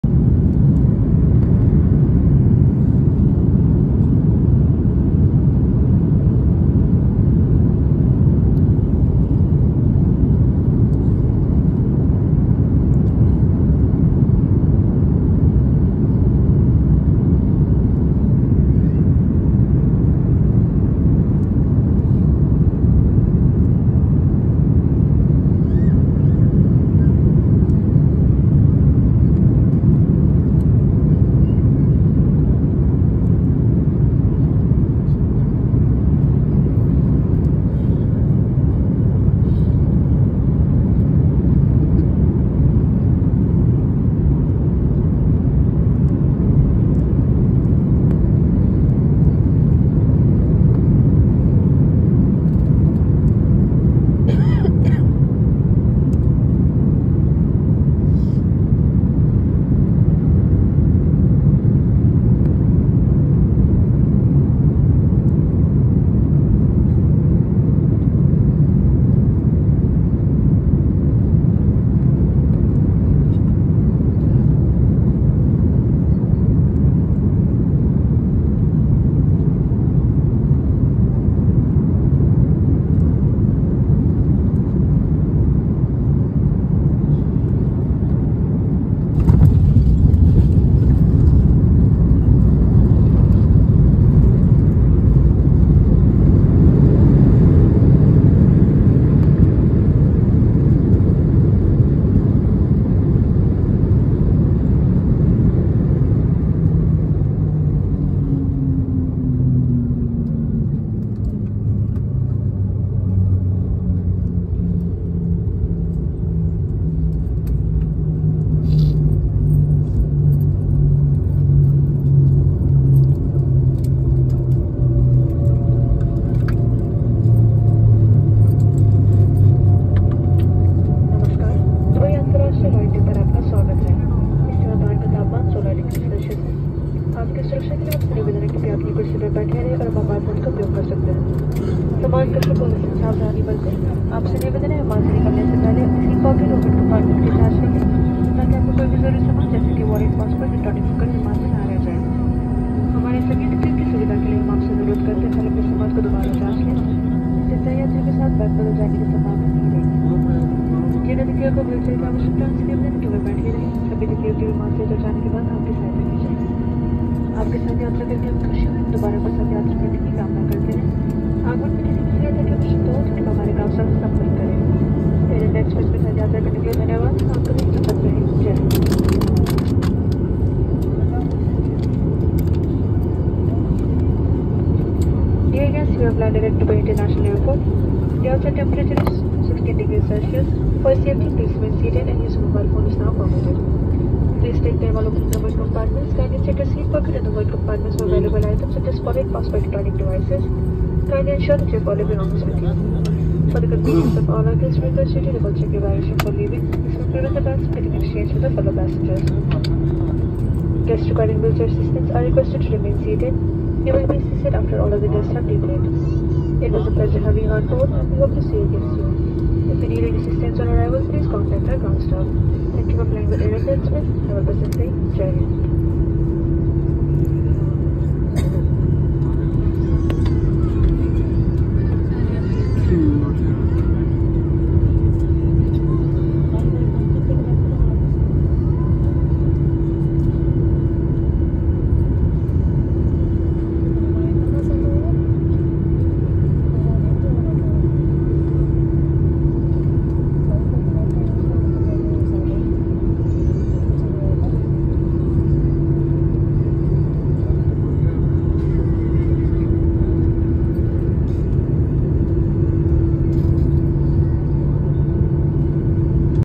INDIA TO DUBAI | Airport | plane | landing and departure from Airport
Air India express flight landing in dubai